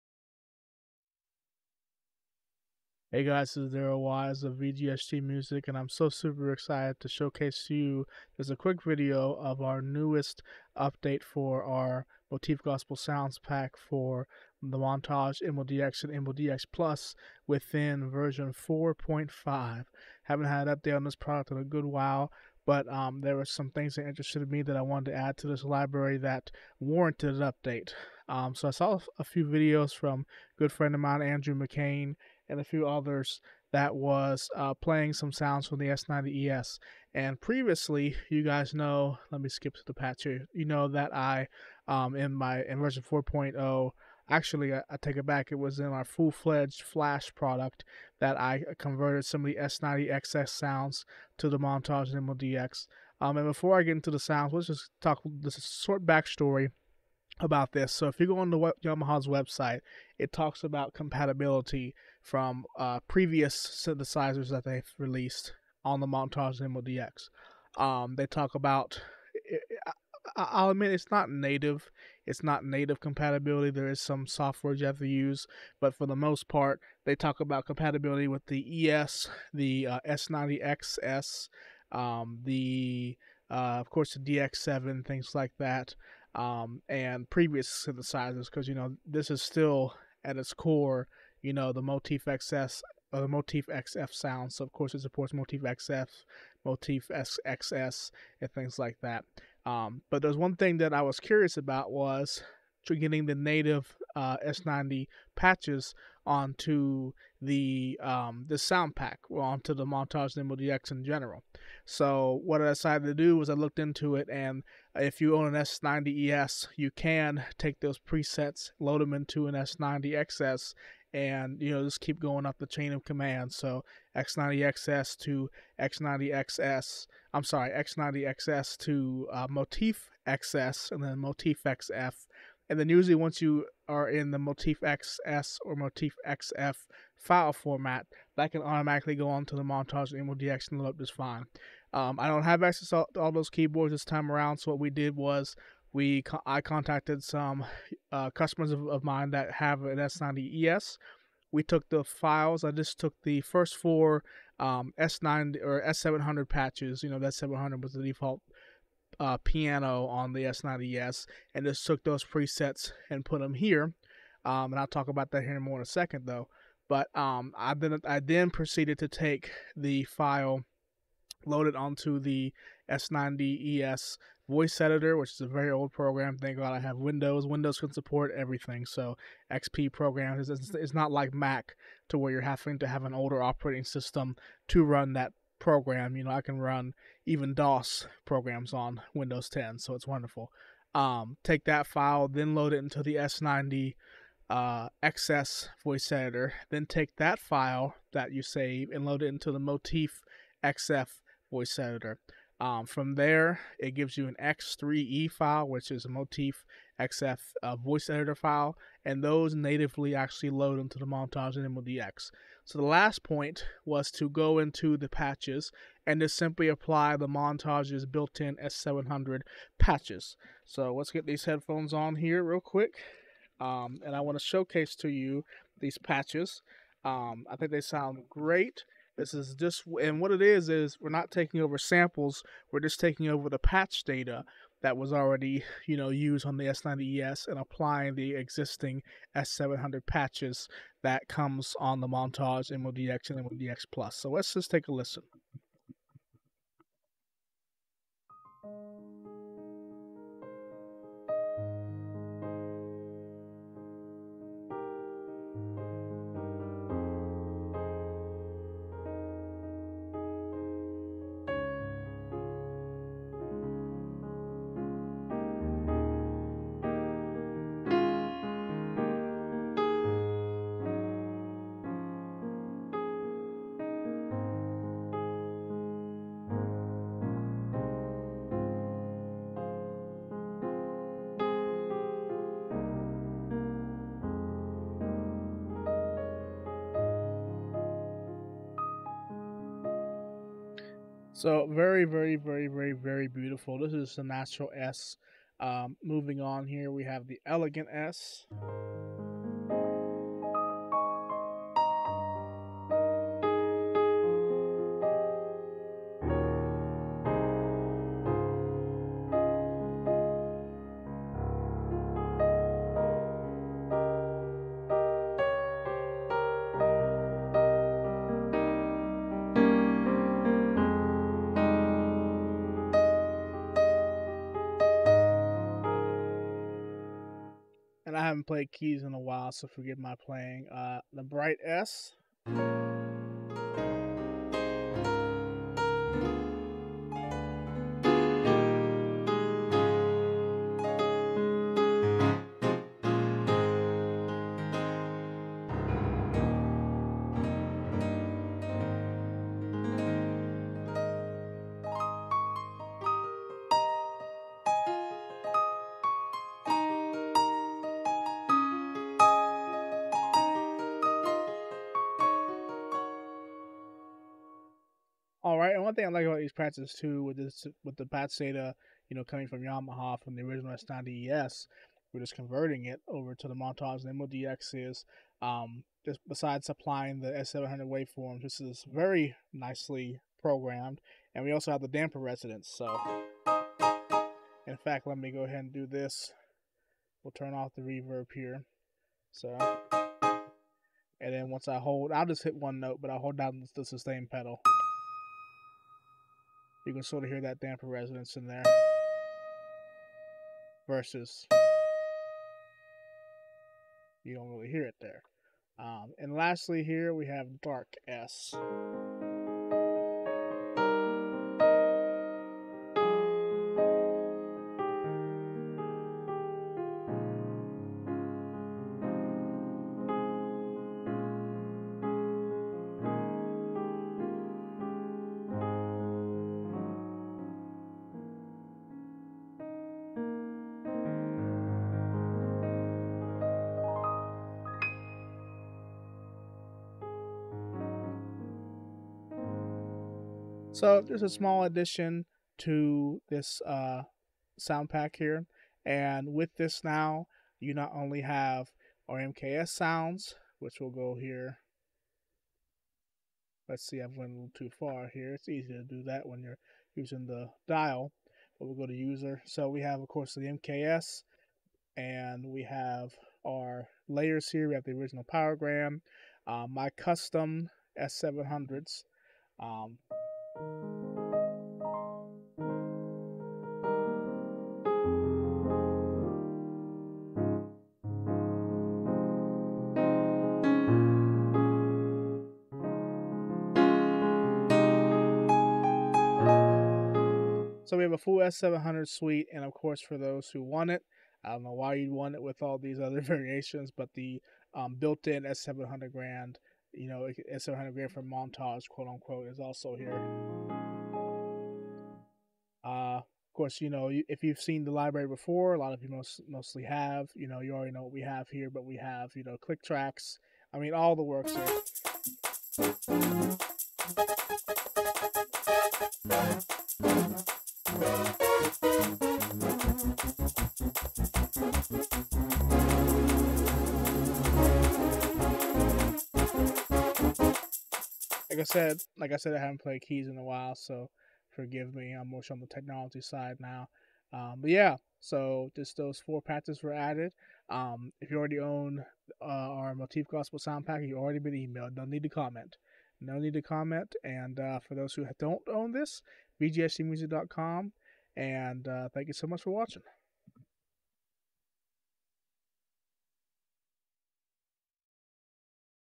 MONTAGE/MODX Gospel Sound Pack v4.5 - Overview
Version 4.5 of our MONTAGE/MODX Gospel Sound Pack adds patch data from the S90ES's first four factory piano presets giving this pack an abundant S700 Piano sound suite!